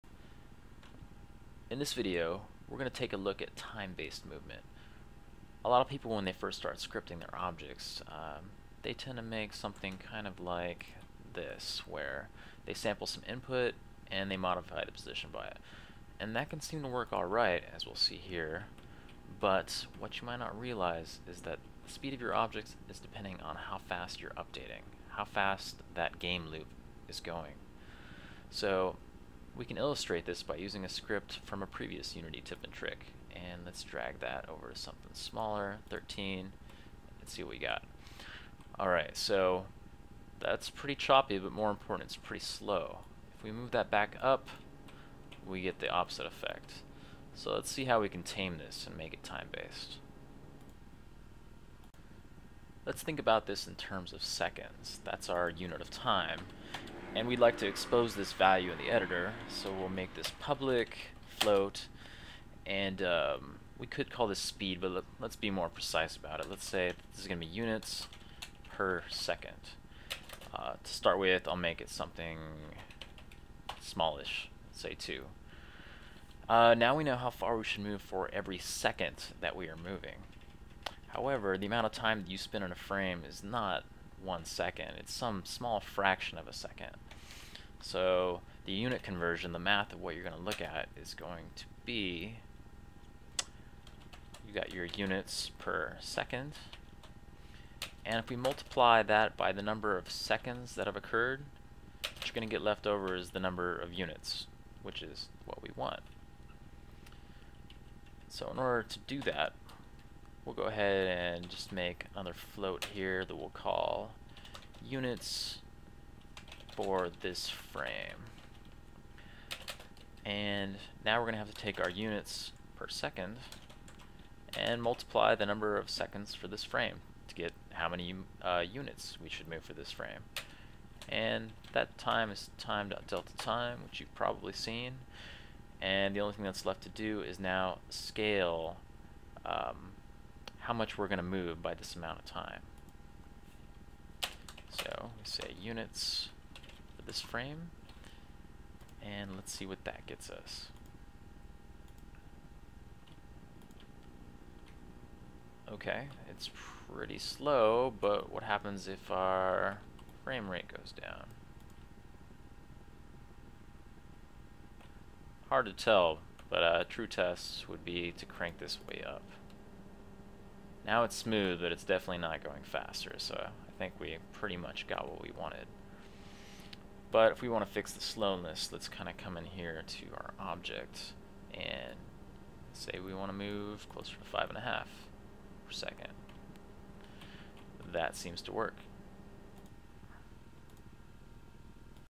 Unity Tips and Tricks: Time-based Movement
The process of converting scripted movement from frame-based to time-based using Unity 3.5.